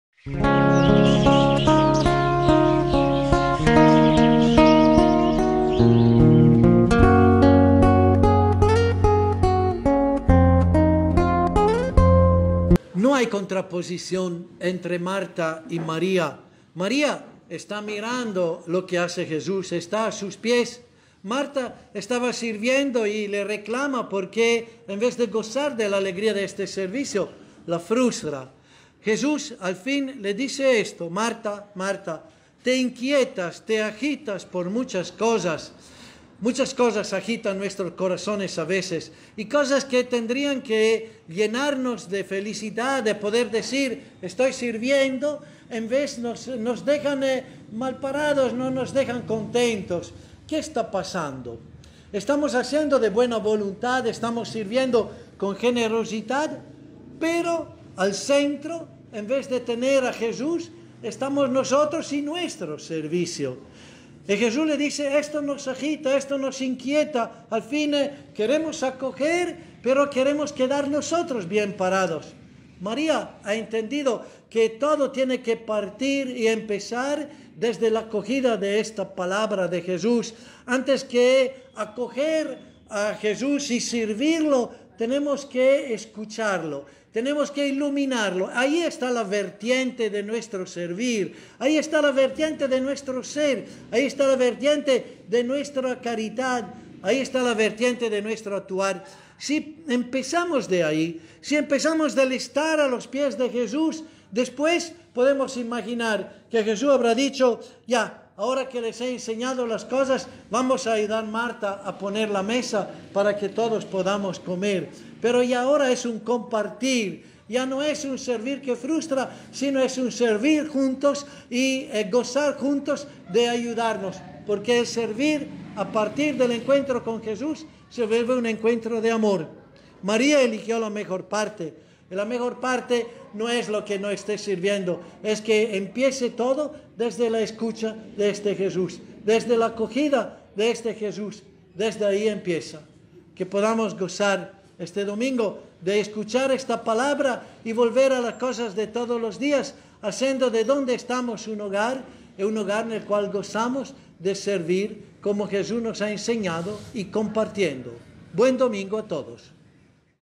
VIDEO: Te sientes vacío a pesar de tus esfuerzos
La respuesta a tu preocupación es que haces todo por ser el centro de atención.
Para servir con alegría y llene tu corazón debes   poner a Cristo al centro y para ello debes primero escuchar la Palabra del Señor que es vertiente del verdadero servicio, caridad y actuar.
Mons. Eugenio Coter.
Gentileza
21 de julio de 2019